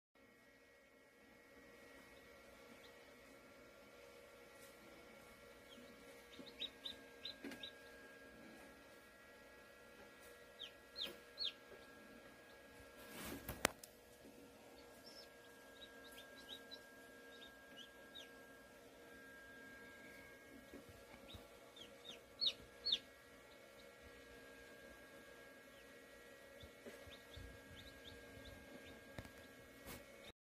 모두건강하게 🍀
1호 시작으로 꼬물이들 부화 스타트 ❤️

❤️  늘즐겁고행복하세요 ❤️